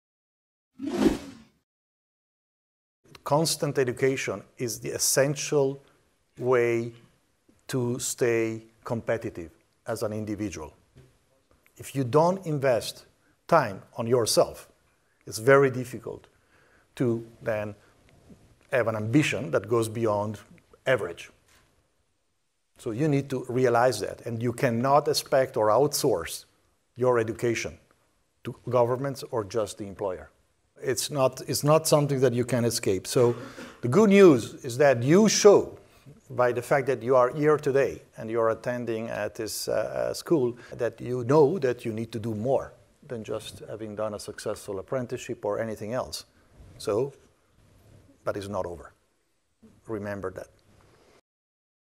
Extrait 2. La Formation continue, un outil indispensable pour rester compétitif
Extrait choisi tiré de la Conférence de rentrée de la HEG-Genève le 20 septembre 2021 en présence de M. Ermotti, président de Swiss Re